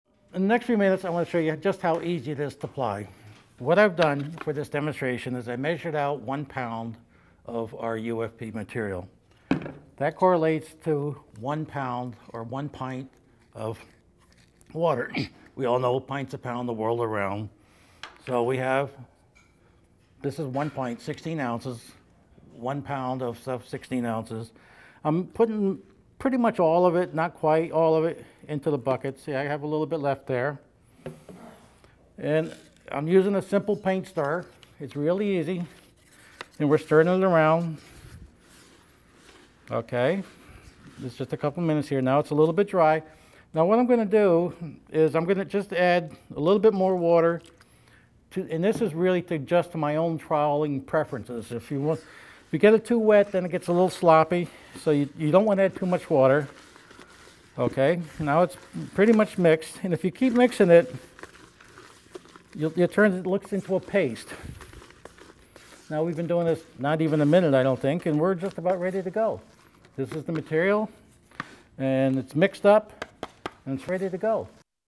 Preparing the Universal Fireproofing Patch.mpg
Vellrath Group has created a state of the art fireproofing patch that can be used in conjunction with existing fire proofing material. It solves the two biggest issues affecting the patch and repair of existing fireproofing.  Specifically, the high cost of patching and repair is reduced by 50% and two, the material is fully vetted by not one, not two, but three nationally recognized, completely independent, testing laboratories.